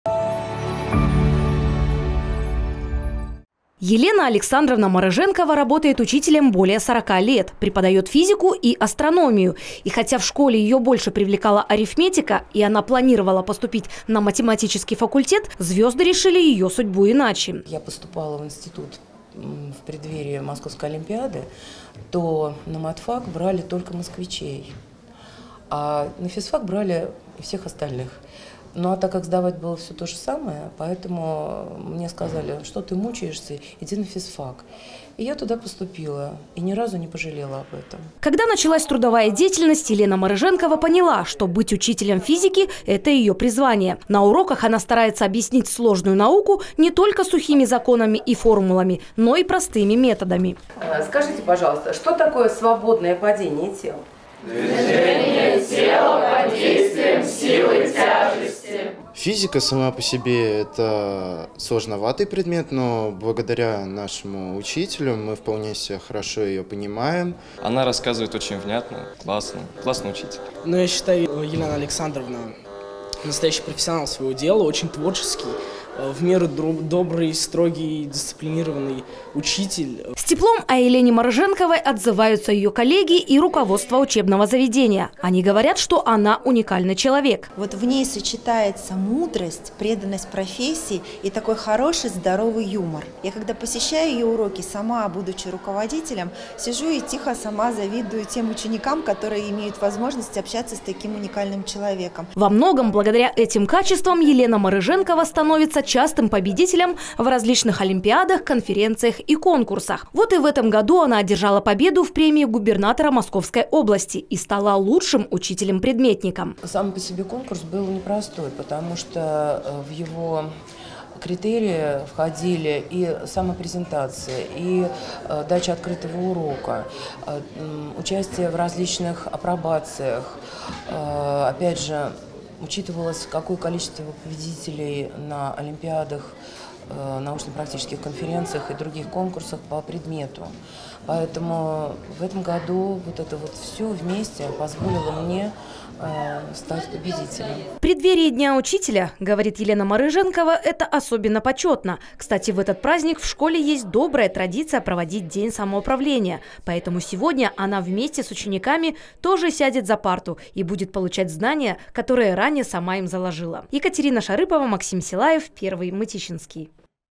Учитель физики школы № 8 Елена Морыженкова стала победителем премии губернатора Подмосковья
Мытищинская школа №8 признана лидером высокого качества образования. На Всероссийском конкурсе «Профессионалы – гордость России» учителя продемонстрировали передовой педагогический опыт. Среди ведущих учителей школы – Елена Морыженкова, преподаватель по физике. Она стала победителем премии губернатора Подмосковья «Лучший учитель-предметник». О жизненном пути, любви к своей школе и успехах в работе рассказываем в рубрике «Мытищи – это МЫ». (12+)